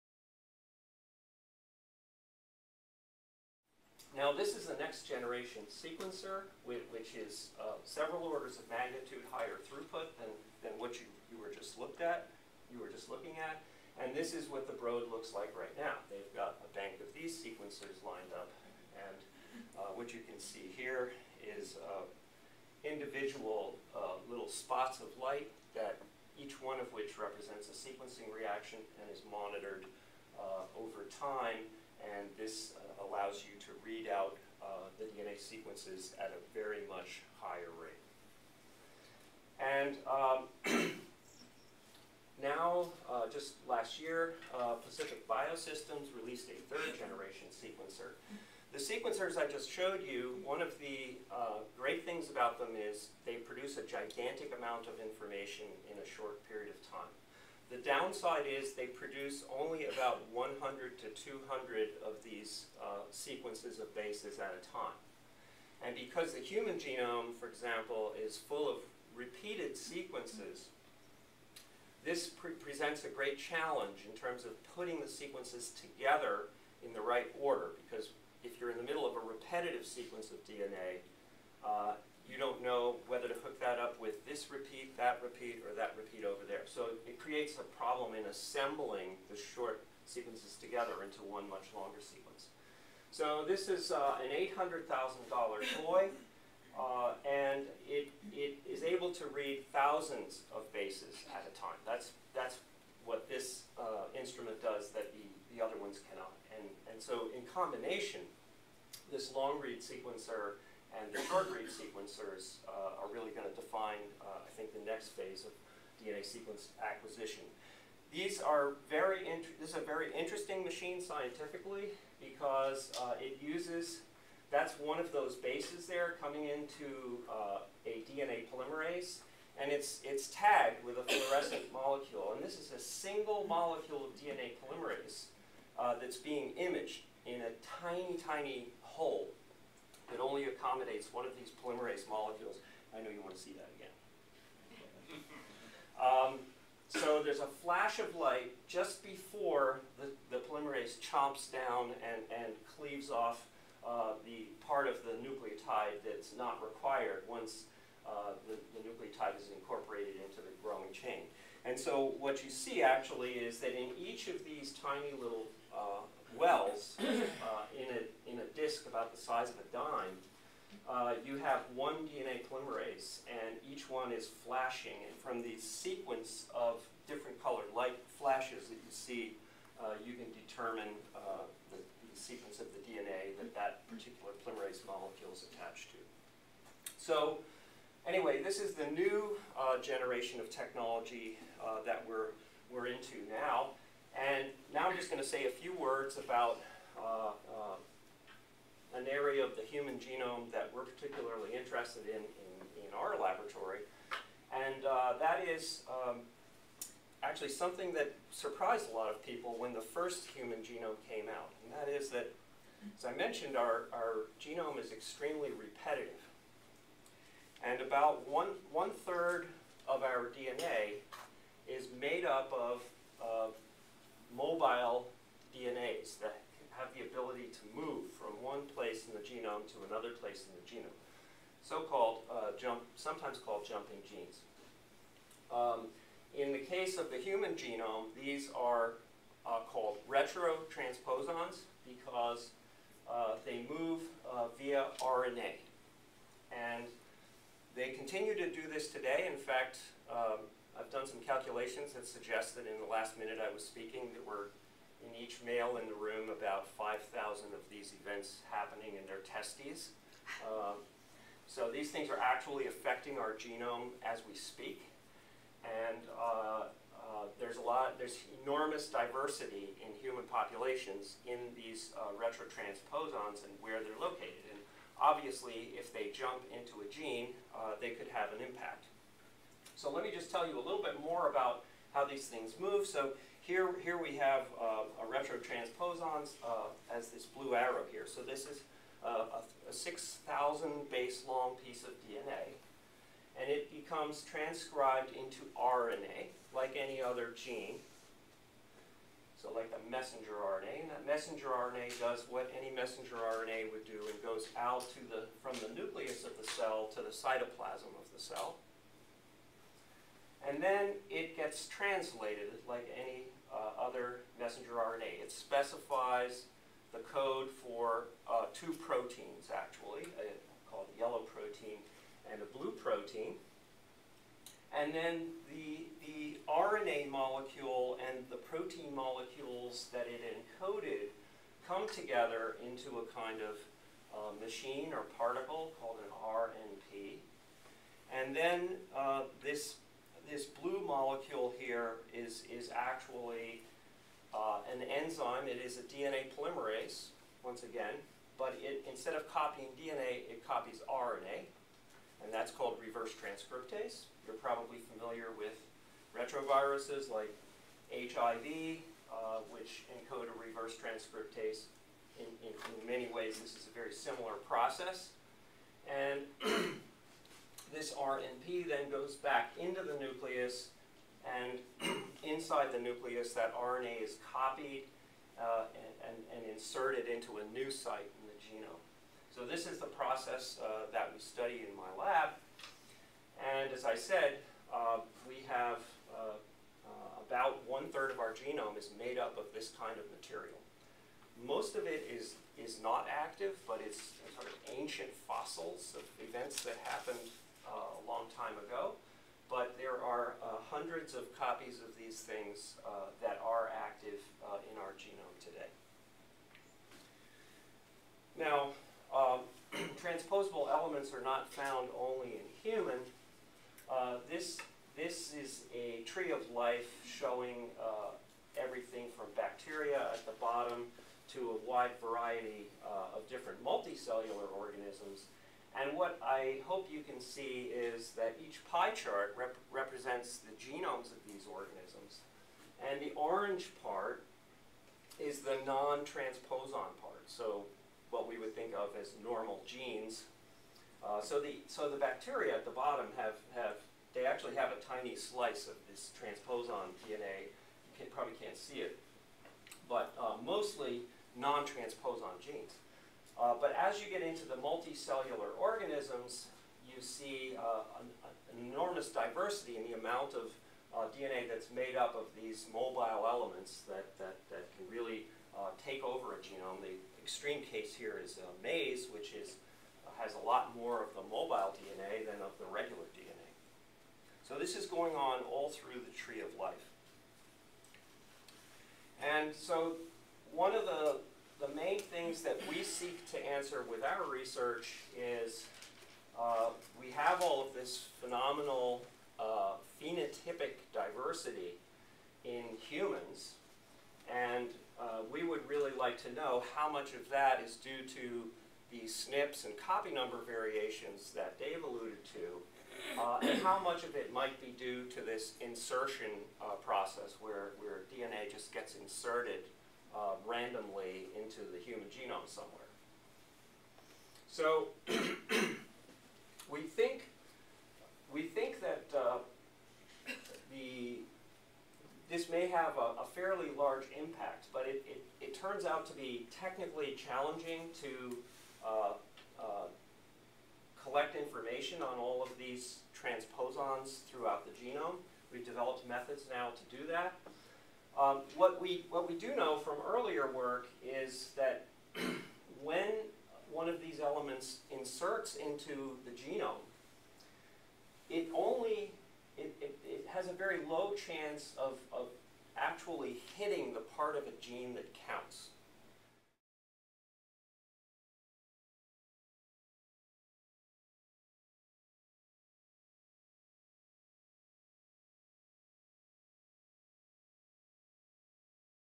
Part II, Talking the Genomics Talk
Jef Boeke's talk at the Genes To Society science writers' boot camp on May 6, 2011